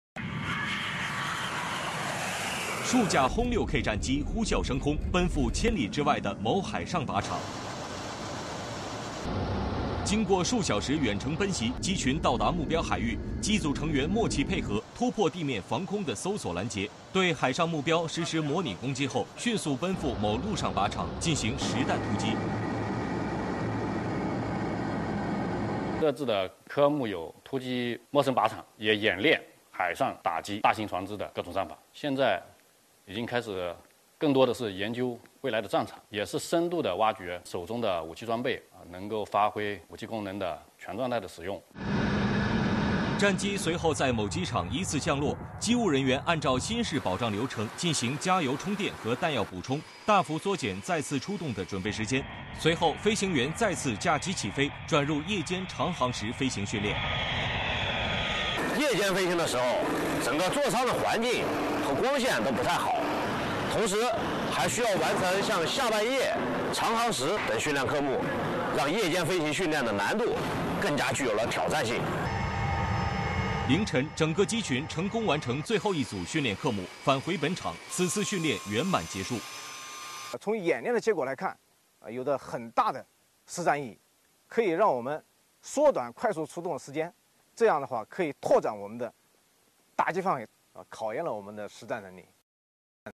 【直击演训场】轰-6k机群跨昼夜突防突击
中部战区空军航空兵某团近日组织轰六K战机展开跨昼夜远程突防突击训练,跨区域机动、夜间长航时飞行、全时段异地机场起降等课目，锤炼了部队远程机动和战场适应能力。（央视军事报道.解放军新闻传播中心融媒体出品）